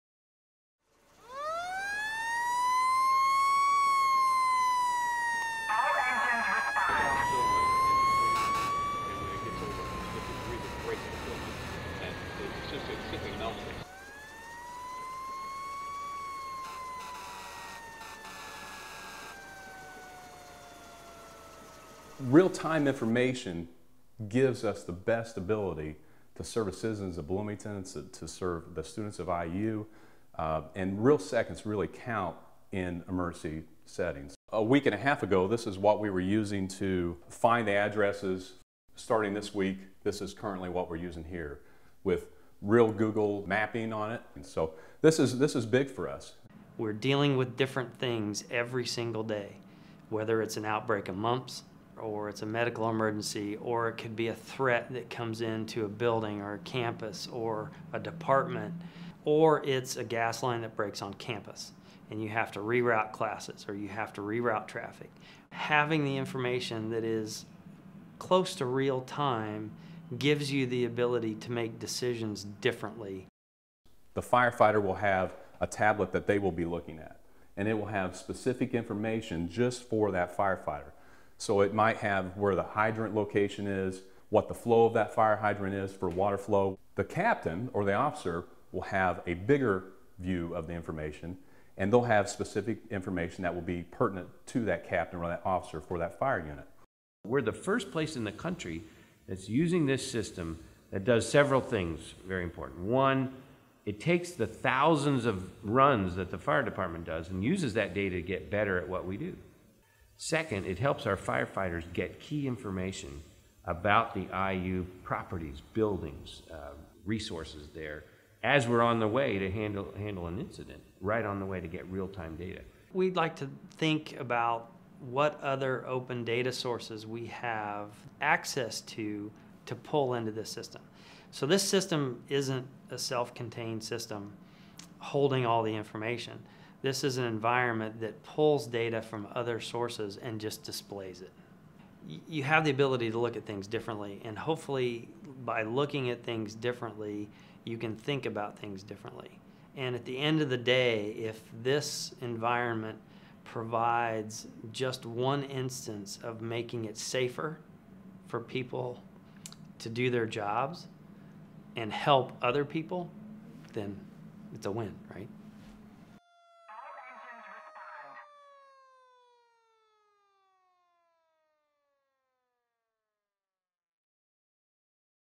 Technology on fire
A new collaboration between the Bloomington Fire Department, Indiana University, and software company Netage uses real-time data to help local firefighters work quickly and with the best possible information. Called “Project Innovation,” the initiative is essentially an IT solution to help visualize 911 fire calls.